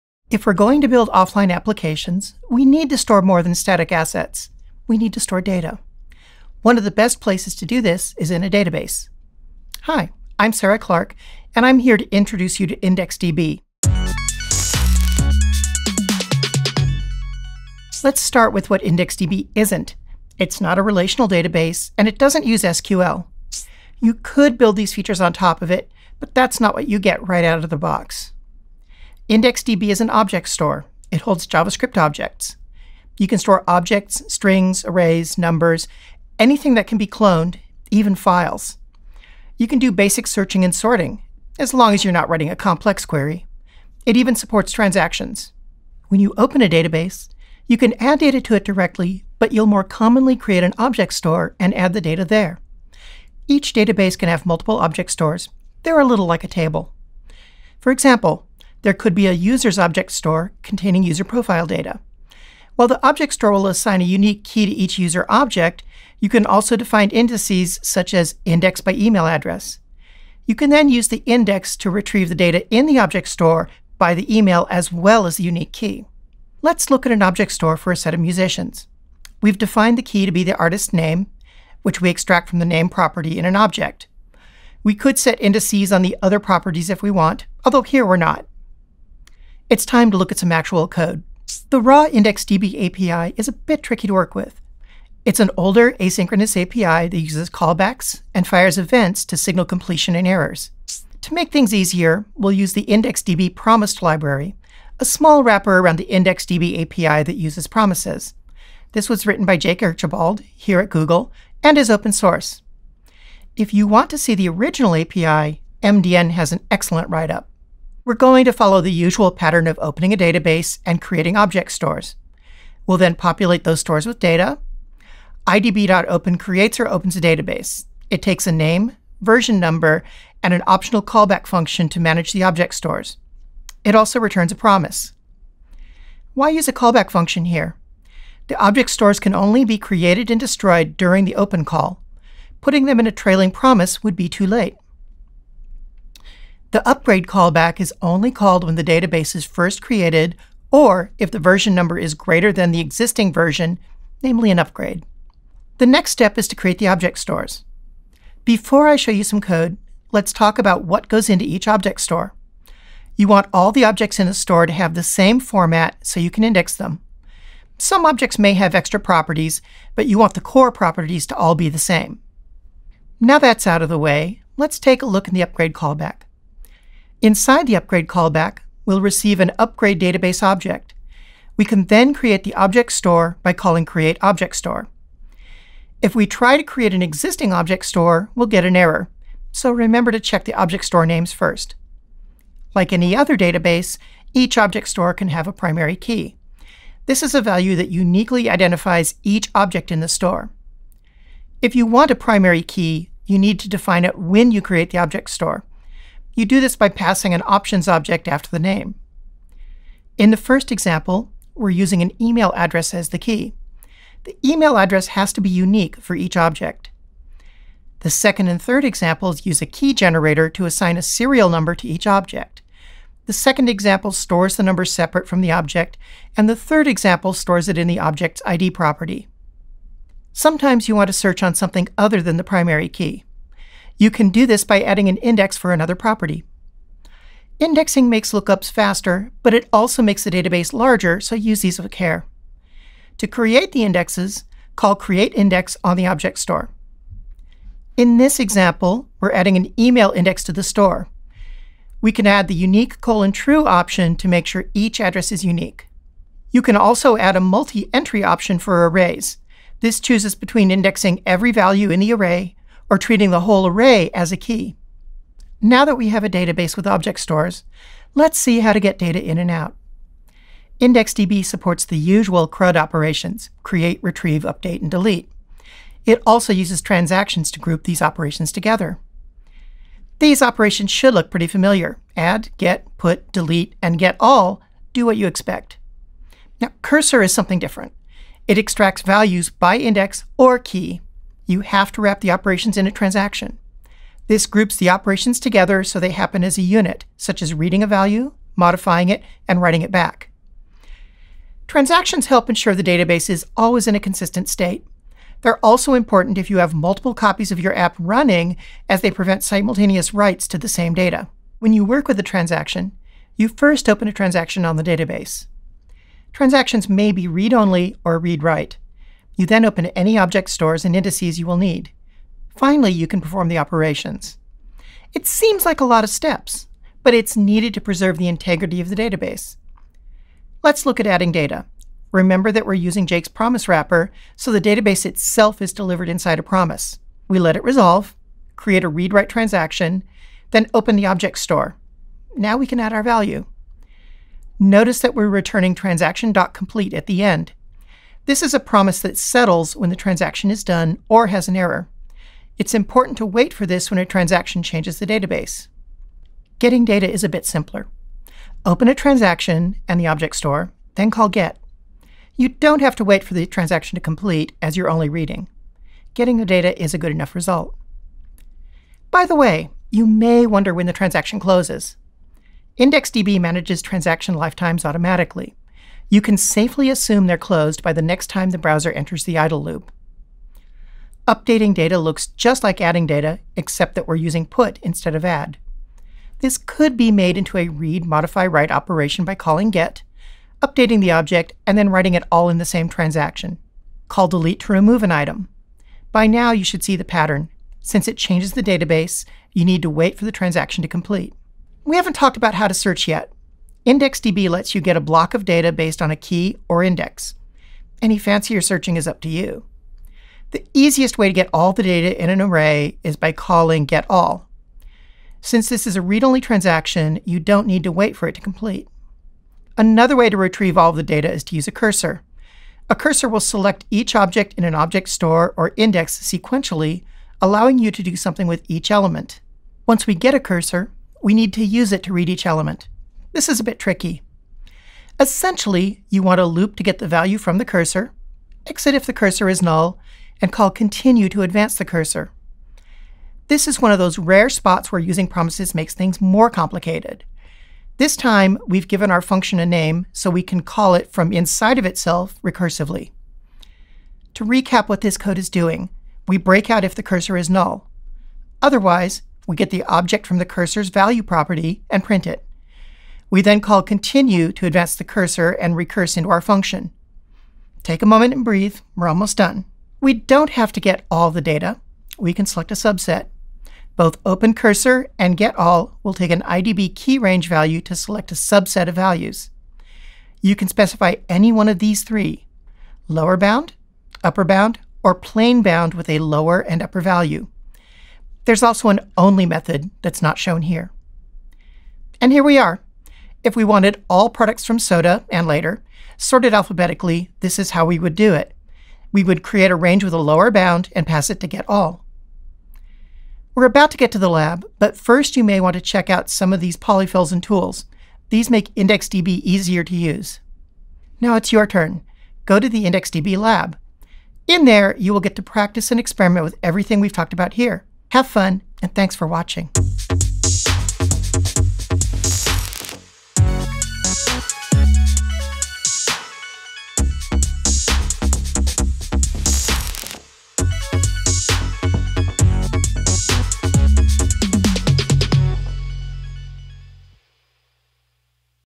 IndexedDB - Progressive Web App Training
For offline applications, you need to store data as well as static assets.

One of the best places to do this is in a database.

This video gives you a simple introduction to IndexedDB, and how to use IndexedDB with Progressive Web Apps.